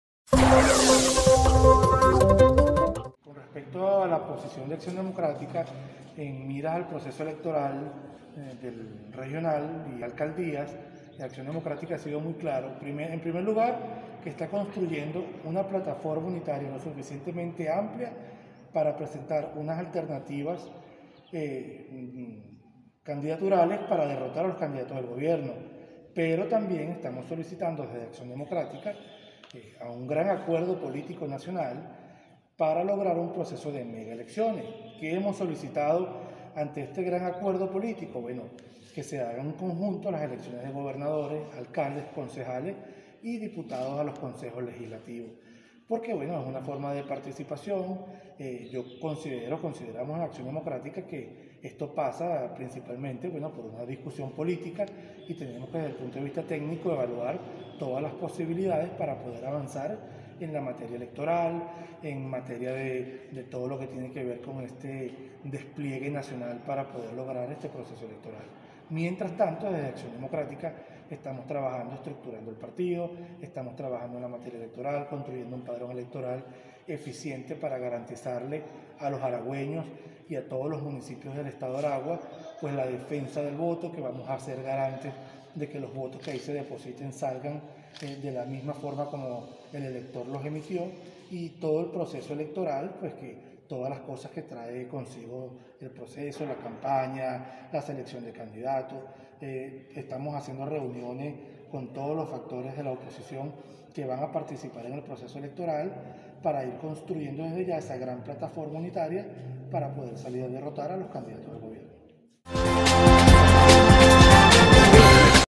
Manuel Guerrero, secretario general de Acción Democrática en el estado Aragua, manifestó que la told
Manuel Guerrero, secretario general de Acción Democrática en el estado Aragua, manifestó que la tolda blanca se encuentra construyendo una plataforma unitaria suficientemente amplia para presentar una alternativa de candidatos para derrotar a los dirigentes del gobierno de Nicolás Maduro en los próximos comicios de alcaldes y gobernadores.